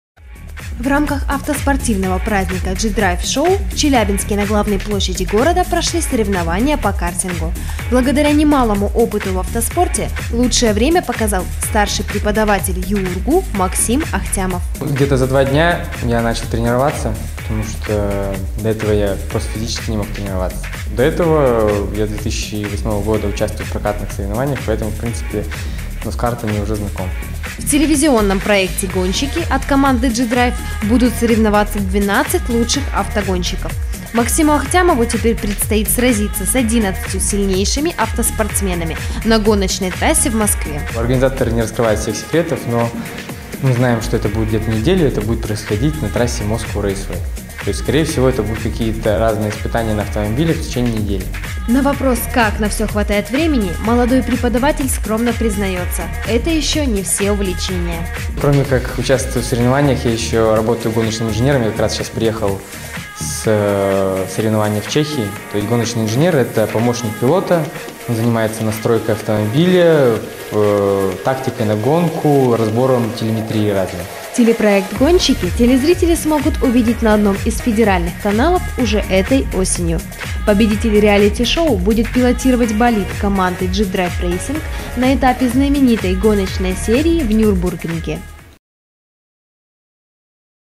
Преподаватель ЮУрГУ примет участие в реалити-шоу «Гонщики».
Старший преподаватель кафедры «Экономика и инновационное развитие бизнеса» примет участие в телевизионном  шоу «Гонщики».  В рамках автоспортивного праздника «G-Drive Show» в

Челябинске прошли соревнования по картингу на главной площади города. Благодаря немалому опыту в автоспорте, лучшее время показал преподаватель ЮУрГУ Максим Ахтямов.
 В телевизионном проекте «Гонщики» от команды «G-Drive» будут соревноваться 12 лучших автогонщиков. Максиму Ахтямову теперь предстоит сразиться с 11(одиннадцатью) сильнейшими автоспортсменами на гоночной трассе в Москве.
 На вопрос «Как на все хватает времени», молодой преподаватель скромно признается – это еще далеко не все увлечения.
 Телепроект «Гонщики» телезрители смогут увидеть на одном из федеральных каналов уже этой осенью. Победитель реалити-шоу будет пилотировать болид команды «G-Drive Racing» на этапе знаменитой гоночной серии в Нюрбургринге.